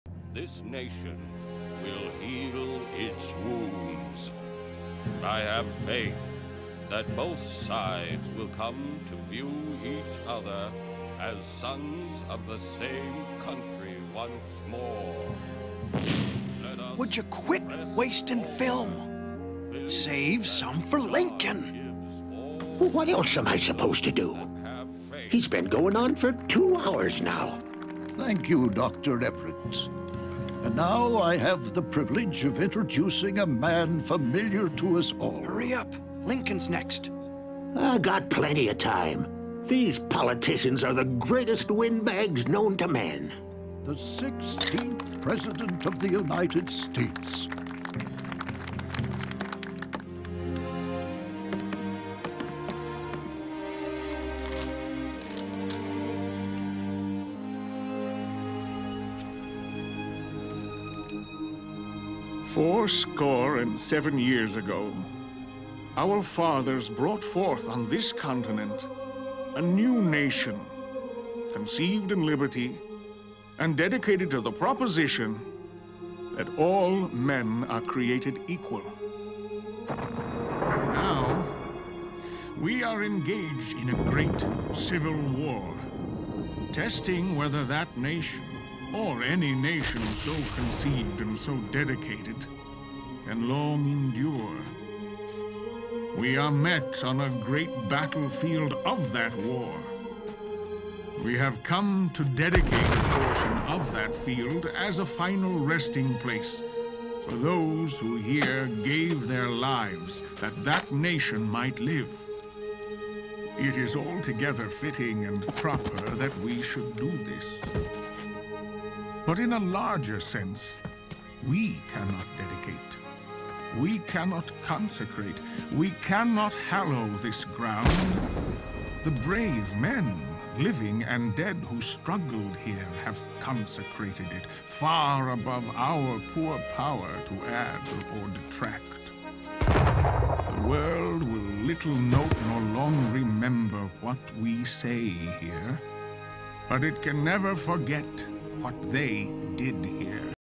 Abraham Lincoln Ch 12
"For My Freedom." Emphasize the importance of political and spiritual freedom as you watch this video clip and learn that Abraham Lincoln, 16th President of the United States, believed in and obeyed God. He knew that slavery was wrong and prayed to God for guidance to lead the United States during his presidency.